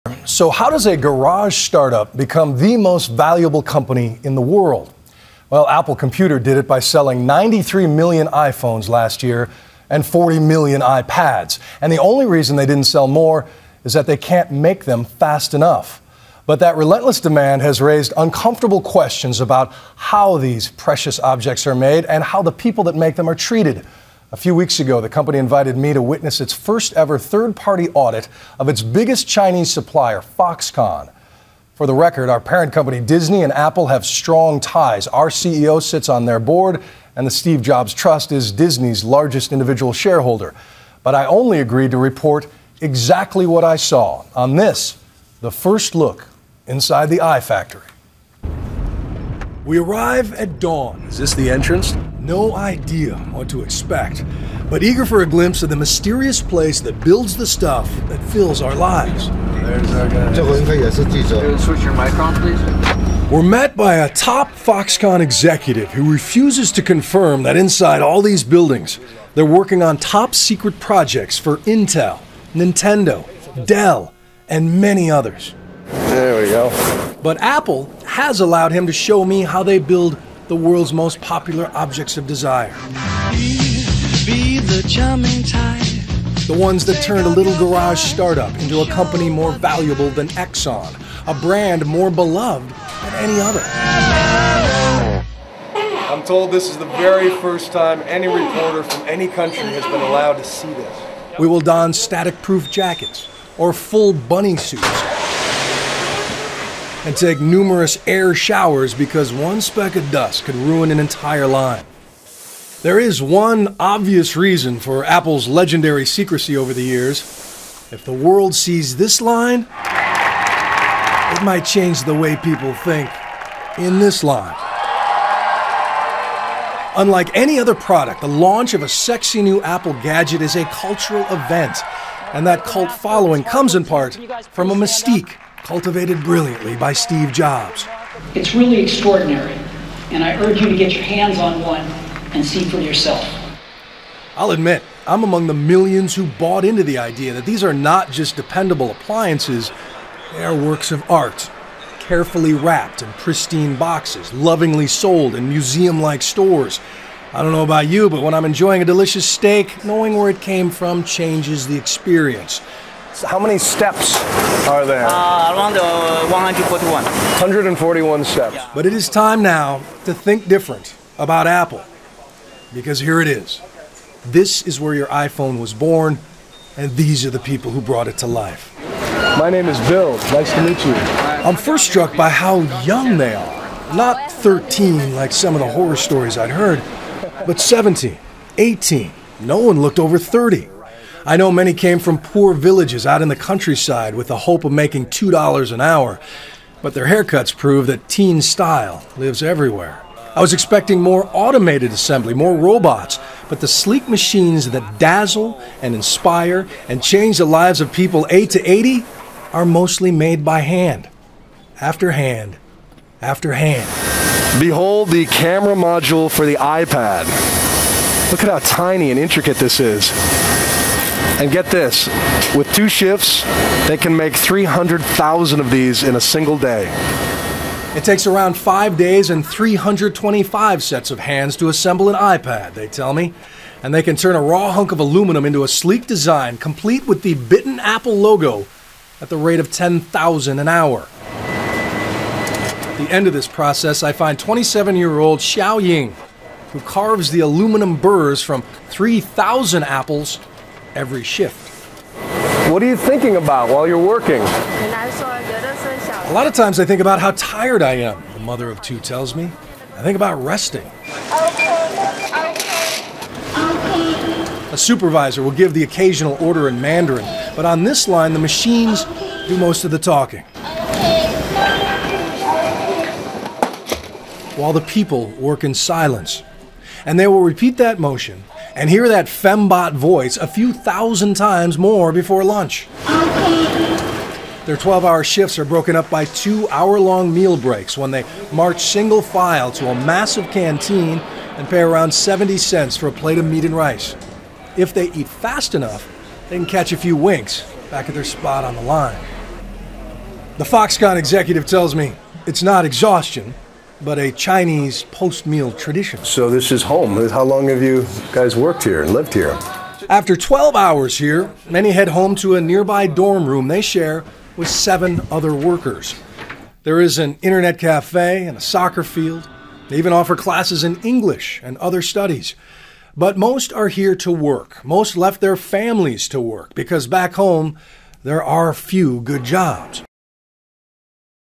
Apple's Chinese Factories: Exclusive
Bill Weir's exclusive report on how our Apple iThings are brought to life.